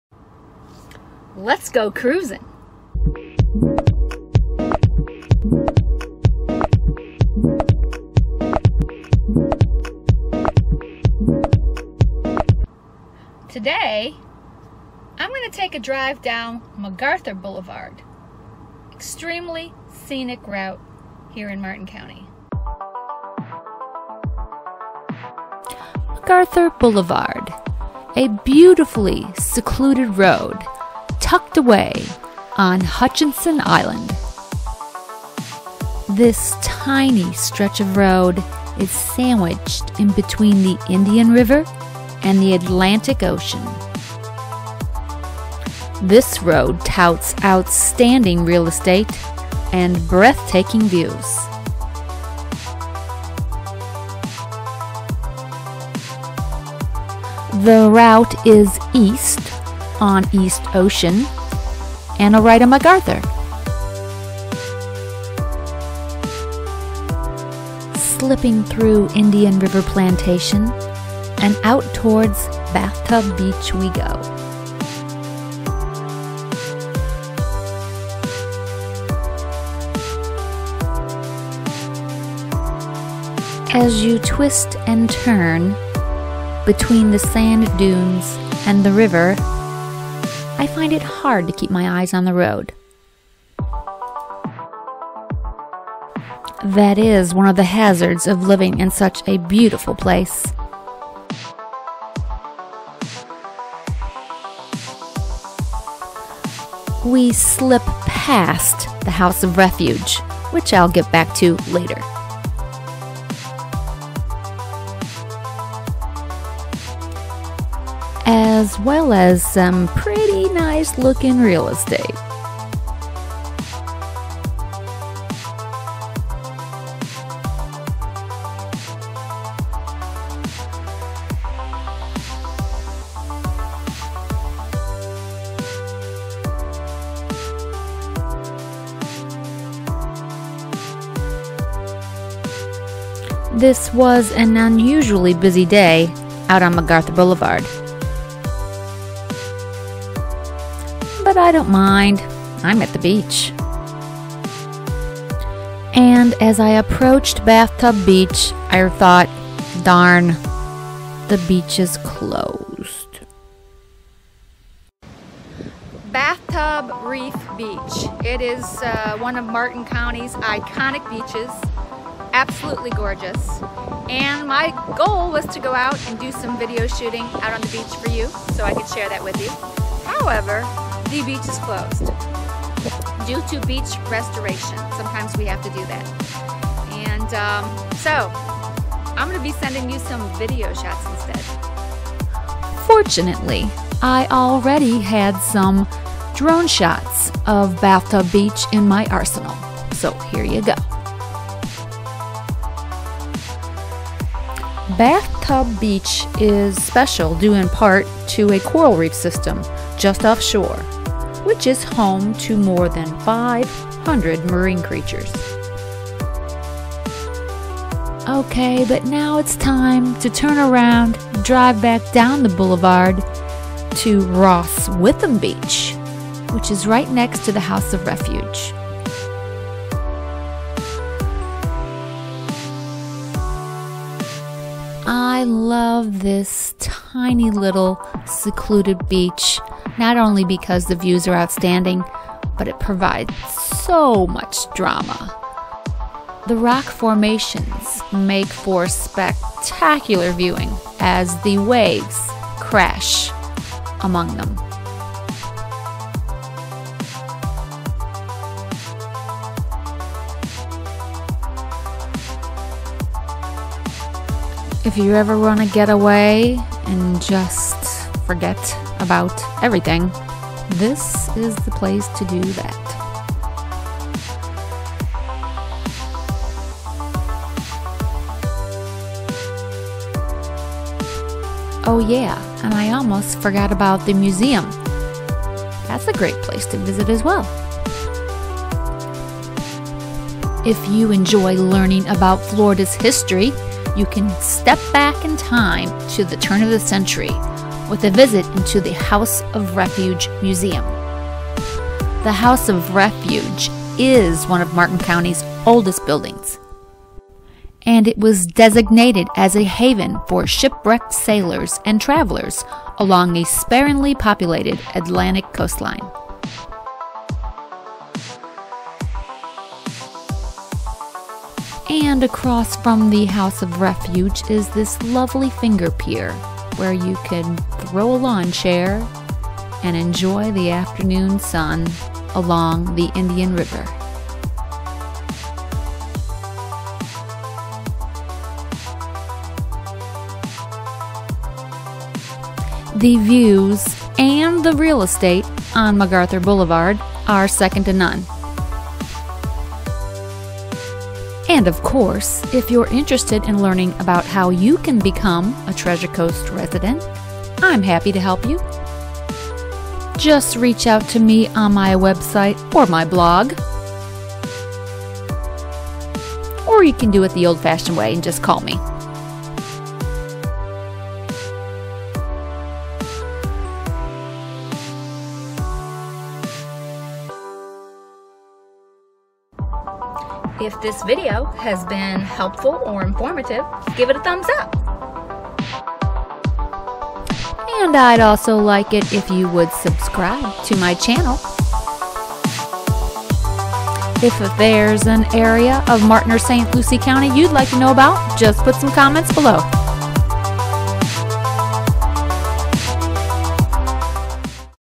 MacArthur Boulevard Stuart Florida - Hutchinson Island
MacArthur Boulevard is a small two-lane road located on Hutchinson Island in Stuart Florida. Stuart  is one of South Florida’s most desirable coastal areas. This little road features both beautiful beaches and beautiful beach homes. If you enjoy a Sunday drive I would put MacArthur Blvd. on your list of things to do.
MacArthur Blvd. is a short road and dead ends at the entrance of Sailfish Point, I personally never get tired of traveling on it even though I have traveled this road at least a hundred times. You have one way in and one way out, if you want to go back to the mainland you need to turn around at Bathtub Reef Beach. The speed limit is 25 MPH and for good reason, as the road does a great deal of winding around and you are in a residential area filled with homes.

As you travel down the road you have the Atlantic Ocean on one side and the Indian River on the other. In some places the road is so narrow you can see the ocean and river at the same time and this can make for a very interesting drive, so keep your eyes on the road.

If you enjoy the beach you will certainly love Bathtub Reef Beach which is one of Stuarts main attractions. This beach is unique, due in part to a reef system just off shore. The reef is home to more than 500 sea creatures and the reef also acts as a barrier from large waves. At low tide the waves cannot get over the reef and this forms a tub like pool in between the reef and shoreline, thus it was named Bathtub Beach.

There are 4 beaches on this stretch of Hutchinson Island and all are open to the public. The second beach I mentioned in my video is Ross Witham Beach and its located right in front of the House of Refuge Museum. Ross Witham Beach has lots of rock formations, so you have a limited ability to get into the water but it has a great deal of beauty.  What I love about this beach is watching the waves hit the rocks and listening to them crash. It can be dramatic at times and provides both visual and acoustic enjoyment.

And while your checking out this beach you might as well check out the house of Refuge Museum. If you enjoy learning about Florida’s history, you can step back in time to the turn of the century. The Houses of Refuge was designated as safe havens for shipwrecked sailors and travelers along the sparsely populated Atlantic coastline of Florida. The museum is loaded with artifacts and photographs of days long past. The House of Refuge is one of Martin County’s oldest building and has been listed on the National Register of Historic Places since 1974 and this museum is certainly something not to miss.

Whether you want to take a break and find a tranquil place on the beach to spend your day or maybe your looking for a home with one of the best views in Florida, taking a drive along MacArthur Boulevard may just be the ticket. I can promise you won’t be disappointed.